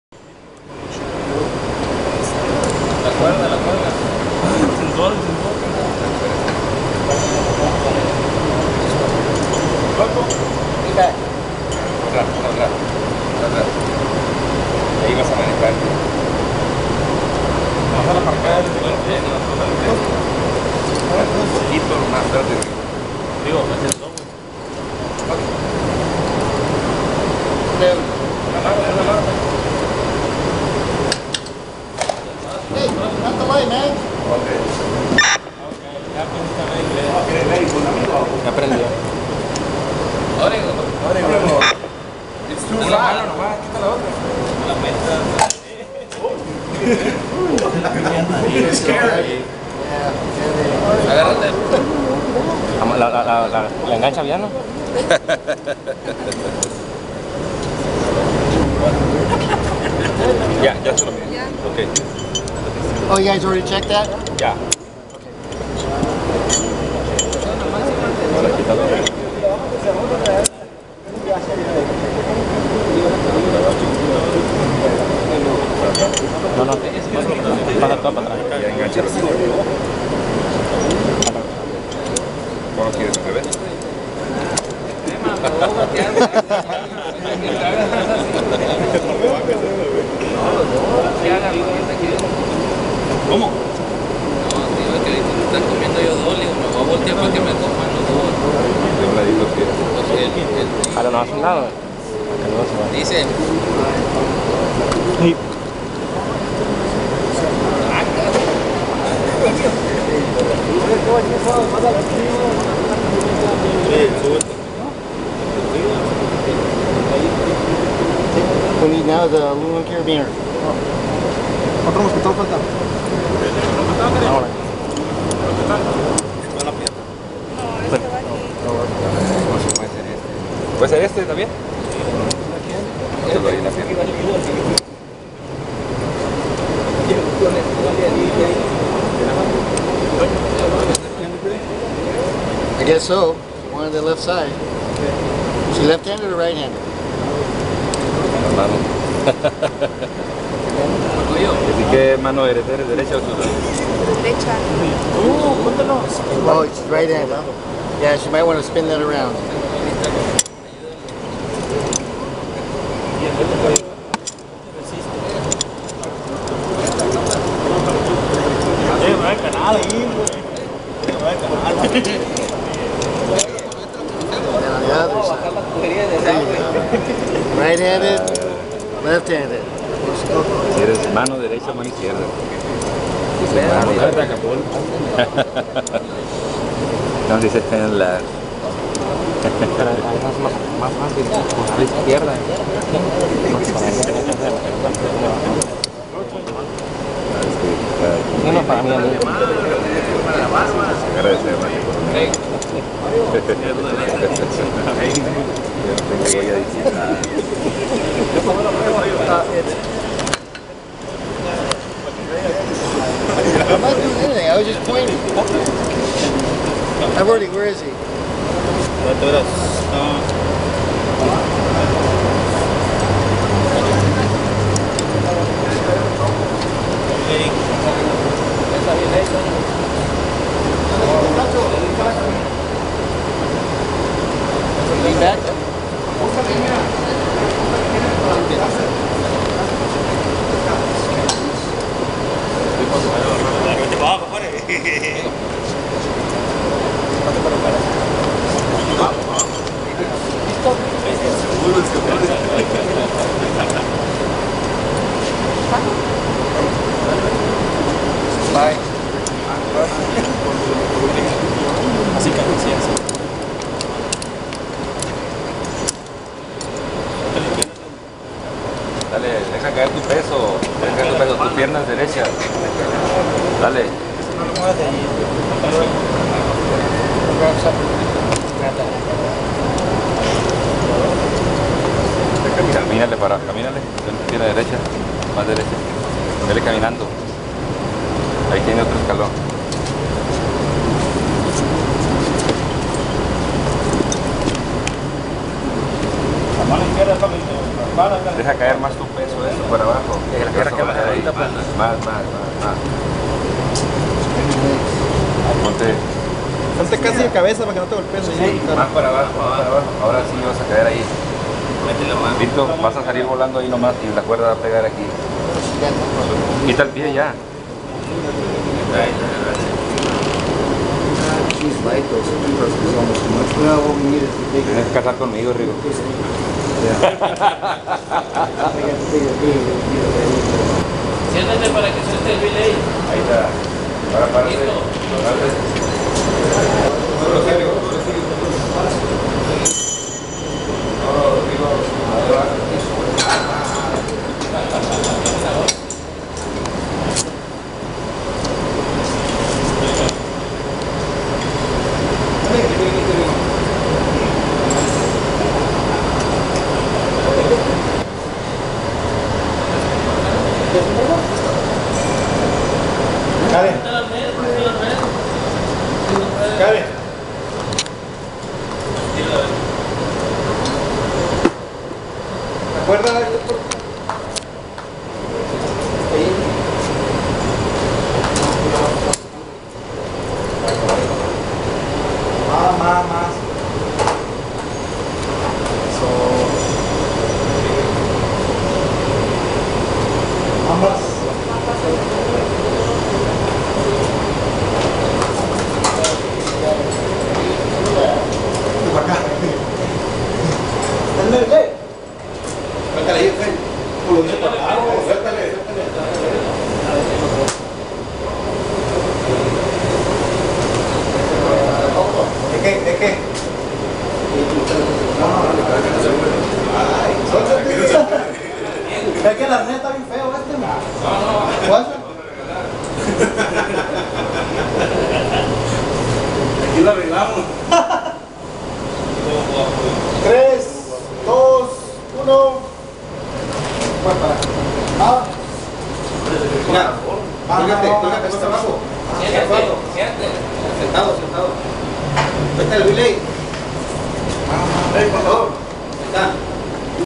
Rescate con cuerdas - 1
El Grupo USAR-Sonora (México) con participación de Bomberos de Hermosillo, Guaymas y Ciudad Obregón, Sonora realizan prácticas de rescate en edificios.-
Instructores: Ernest Ojeda, USAR-USA Chairman and Rescue Captain, Greg Terry, USAR USA Equipment Captain, Ron Klamecky, USAR-USA Captain.
Fecha:Junio 4 al 8, 2012
Lugar: Ciudad Obregón, Sonora, México